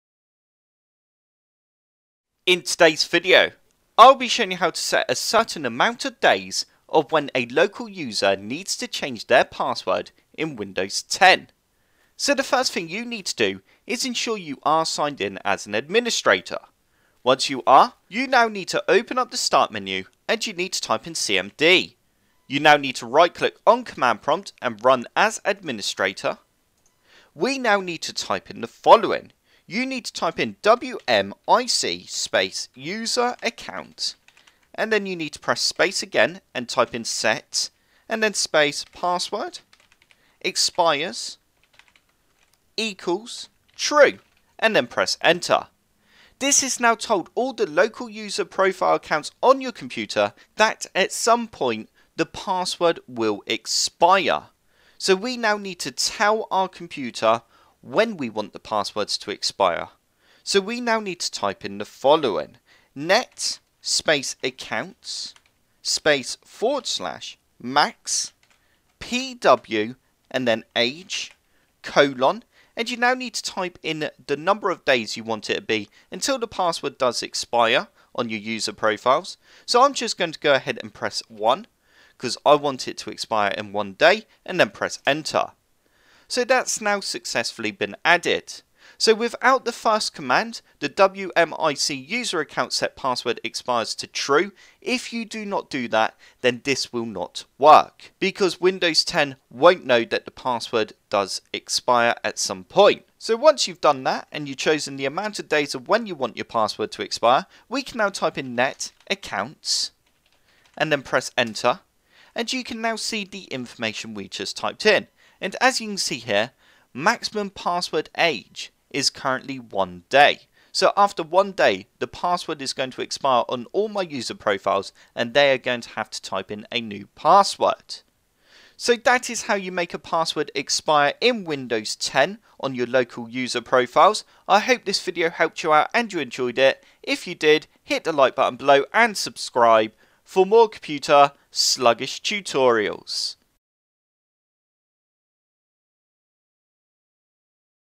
Windows 10 Make Local User Password Expire
Windows 10 Make Local User Change Password After Set Time

In this video, I will be showing you how to make a local user accounts password expire within Windows 10. If you have a few different user profiles on your computer and they all have passwords, then you might want to set a certain amount of days until they have to change their password in Windows 10. You can easily do this using CMD and once it has reached the number of days that you input, then the next time the user signs into Windows 10 they will be asked to change their password.

Code used in cmd:
wmic useraccount set passwordexpires=ture
net accounts /maxpwage: Number Of Days
net accounts